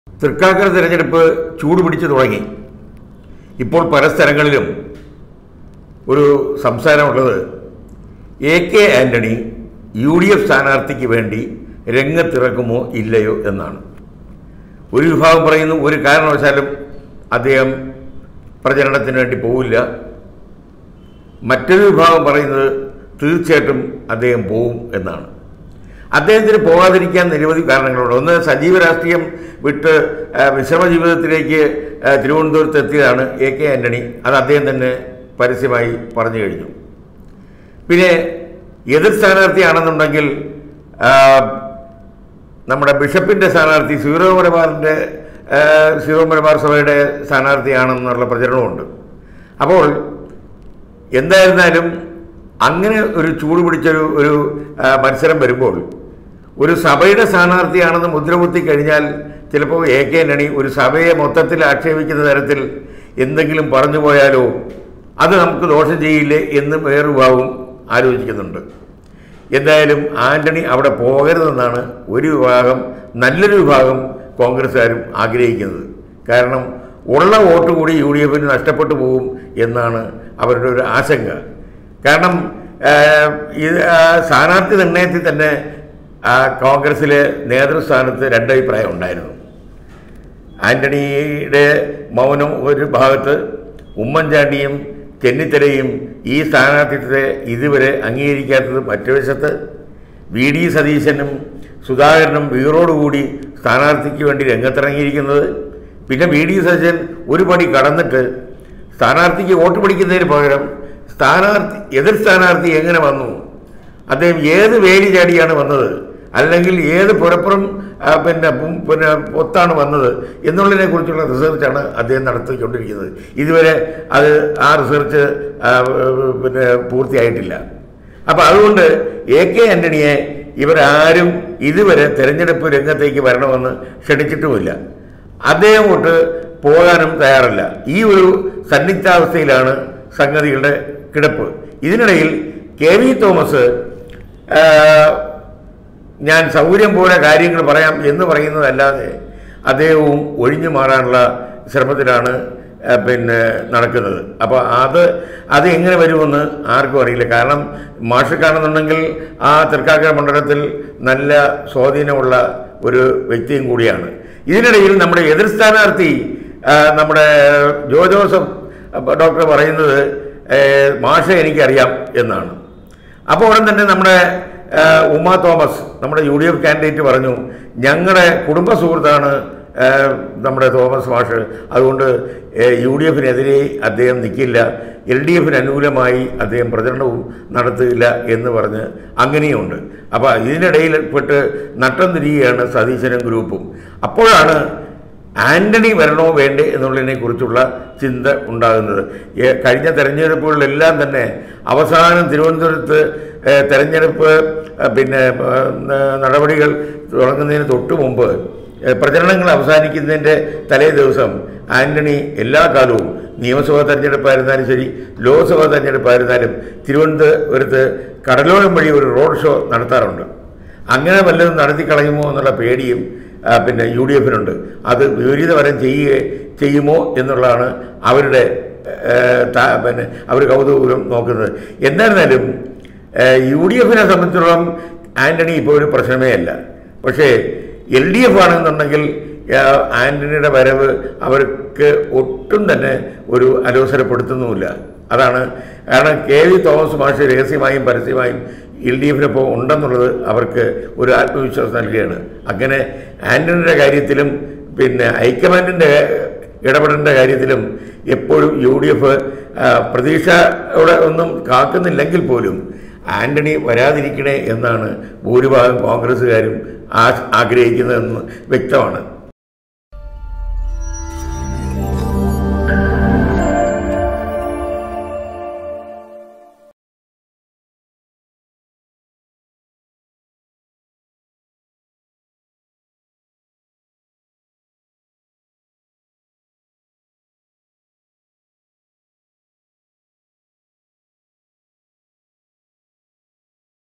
എ കെ ആന്റണി തൃക്കാക്കരയിൽ വരികയോ വരാതിരിക്കുകയോ ചെയ്യാം
Tag-Generator-Youtube-malayalam news-global